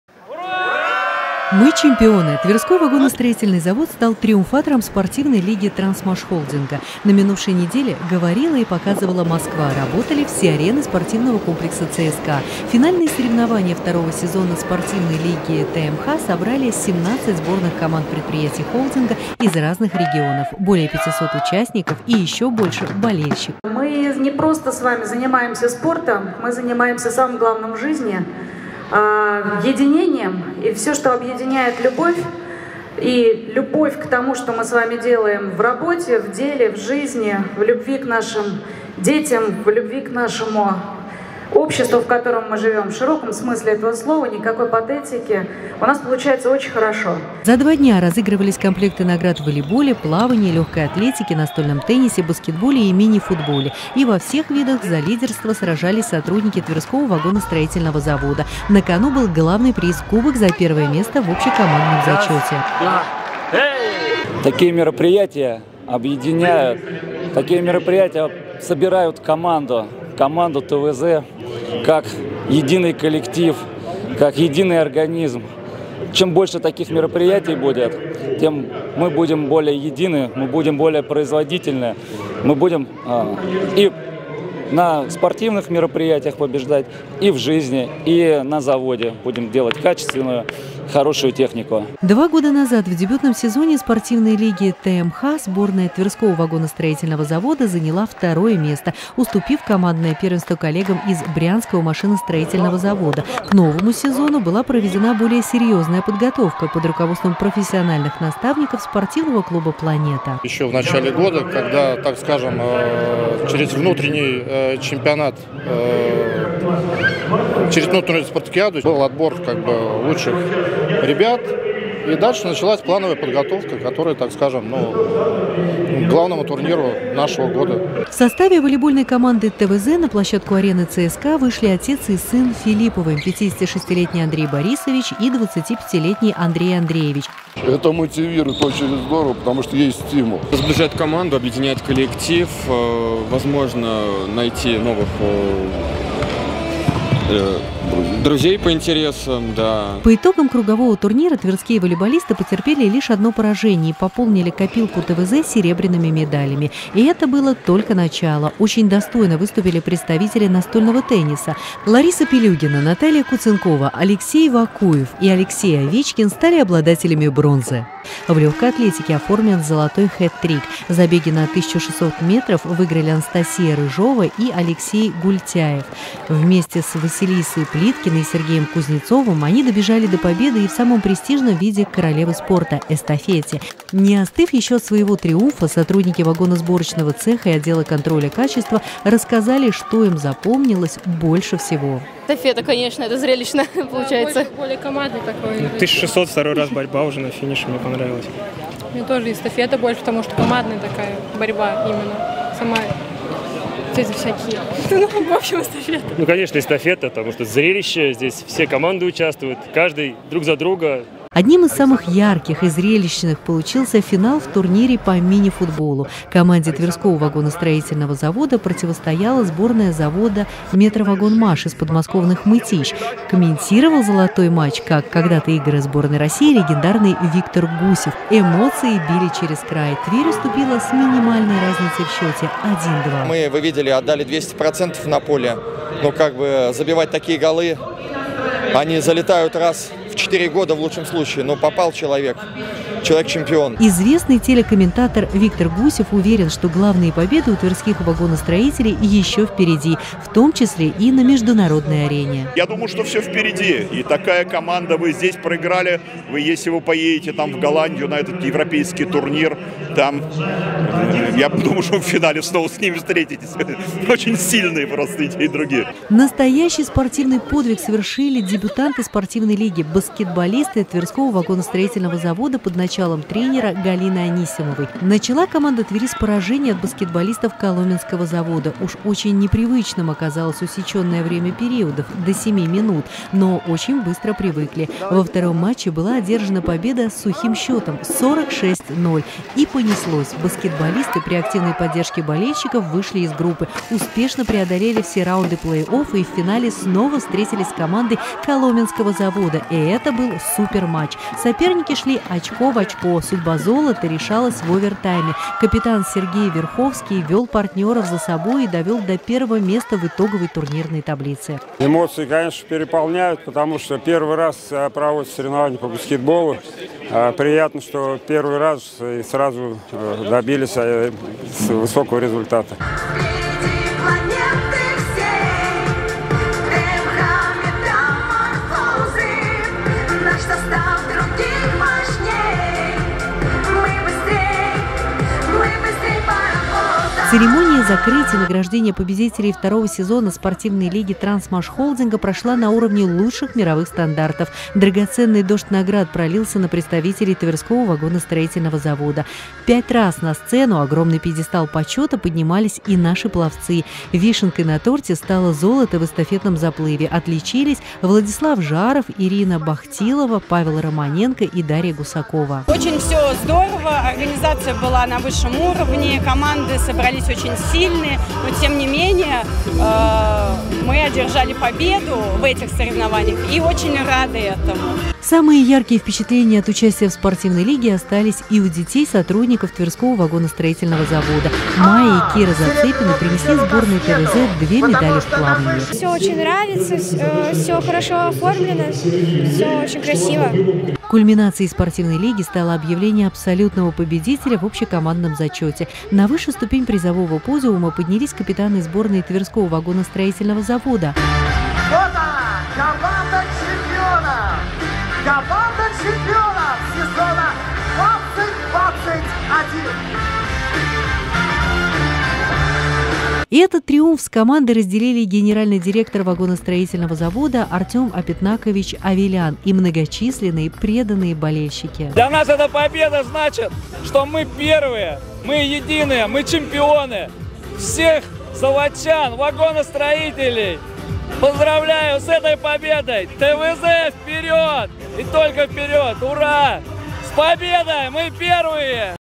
Тверской вагонзавод стал победителем Спортивной лиги ТМХ
В Москве завершился Финал Спортивной лиги ТМХ в сезоне-2020/21. 17 сборных команд предприятий холдинга спорили за награды в шести видах сборная.  Спортсмены Тверского вагоностроительного спорта стали призерами во всех видах и завоевали самый престижный трофей Лиги - кубок за первое место в общекомандном зачете.